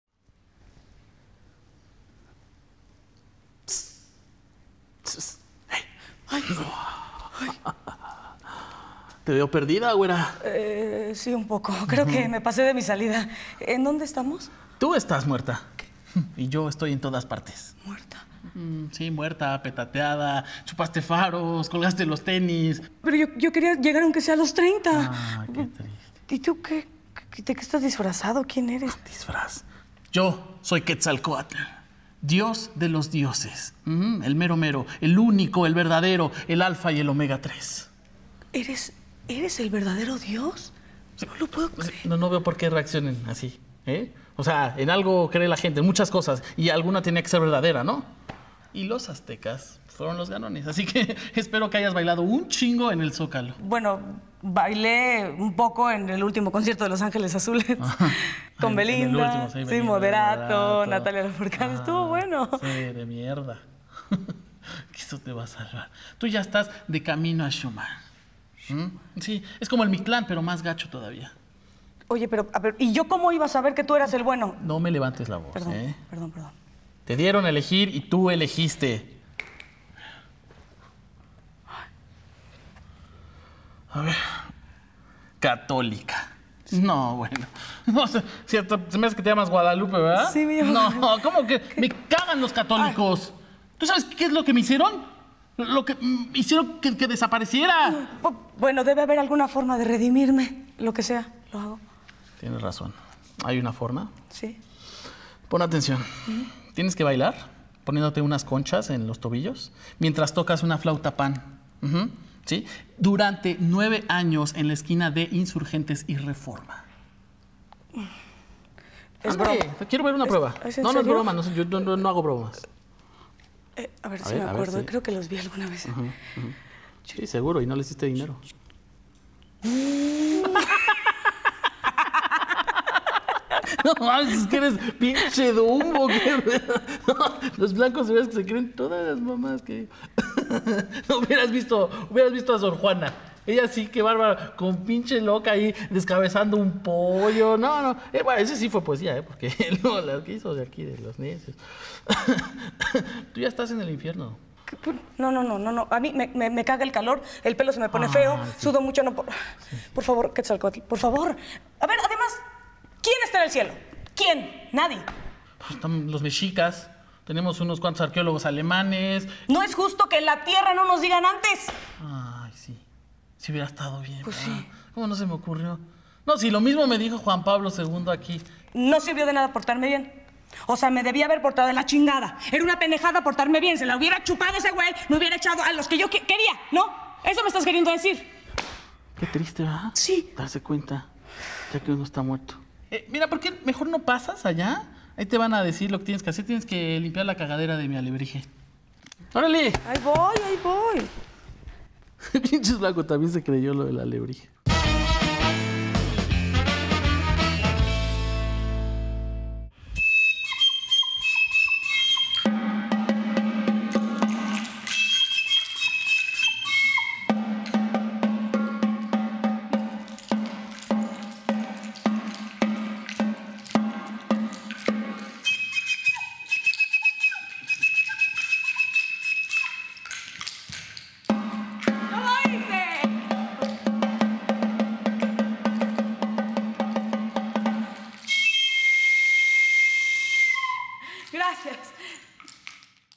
EL VERDADERO DIOS.
Ya me leí un libro que cambio mi vida, en sus dos versiones y en ningun lado me explicaron que tenia que usar conchas en los tobillos, trepar una pirámide el 21 de marzo y ver un calendario que no entiendo. ☝️
Si alguien no hubiera llegado con su viruela desde España, todo sería mejor.

CAST: Daniel García / Luz Aldán

🎬: Salvador Espinosa
✍️: Fabio Porchat
📋:  Gus Proal / Nuria Ocampo/ Renato Guillén/ Miguel Burra / Miauricio Jiménez

Este es el canal de humor Backdoor.
Subimos videos nuevos todas las semanas!